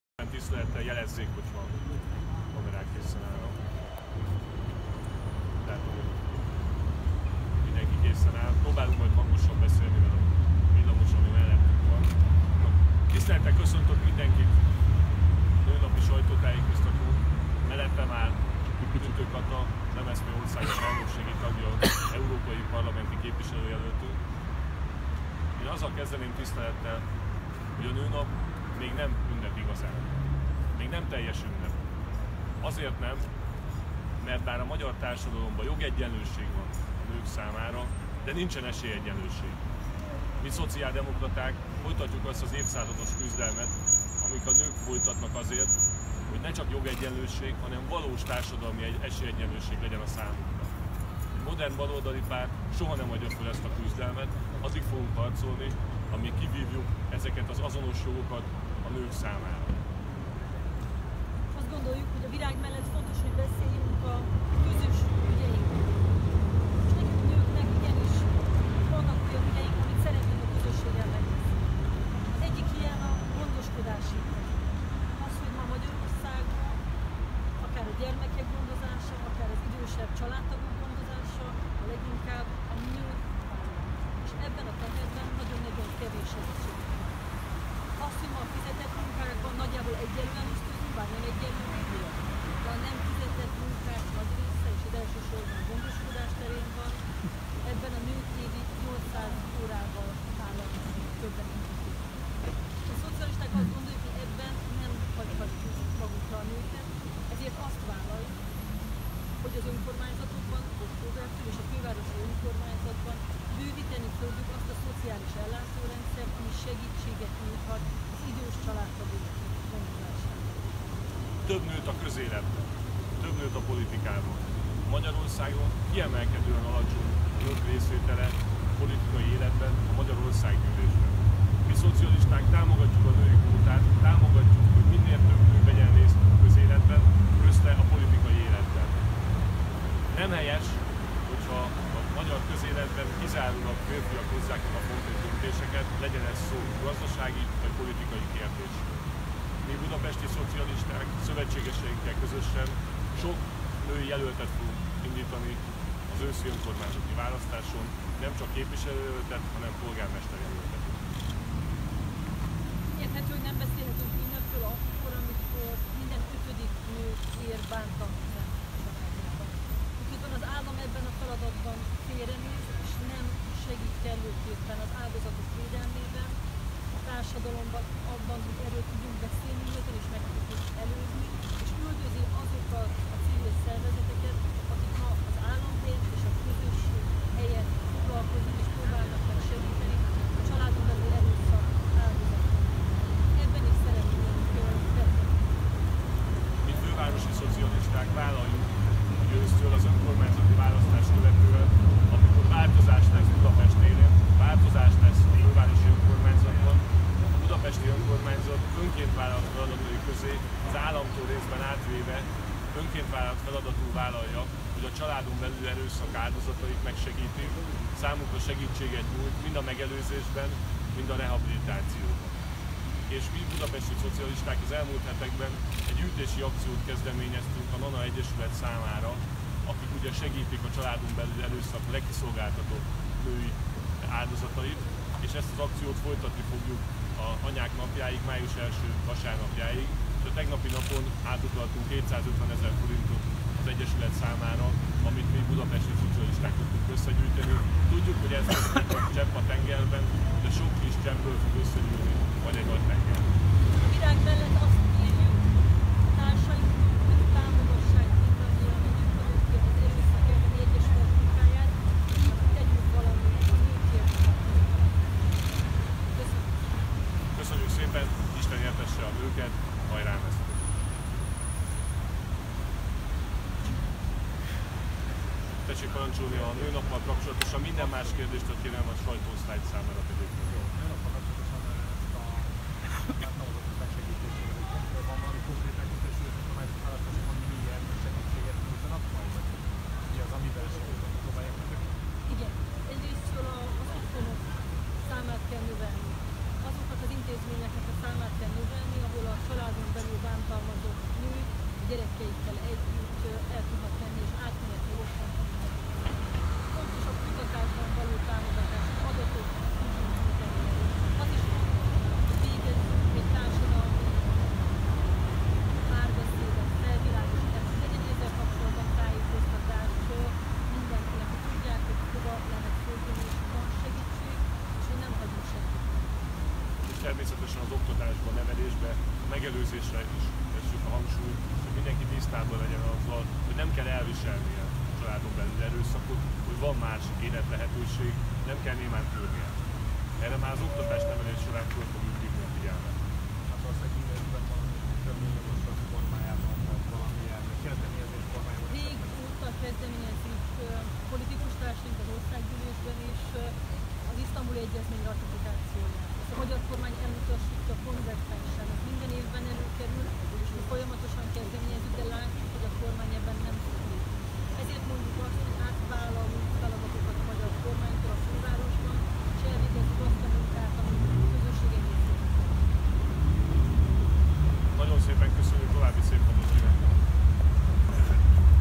2019. 03. 08. Nőnap: a gesztus mellé tettek is kellenek - MSZP
- Molnár Zsolt és Tüttő Kata sajtótájékoztatója

💻 Facebook oldalam, ahol rendszeresen megosztom a fontosabb élő videókat, esetenként híreket osztok, stb: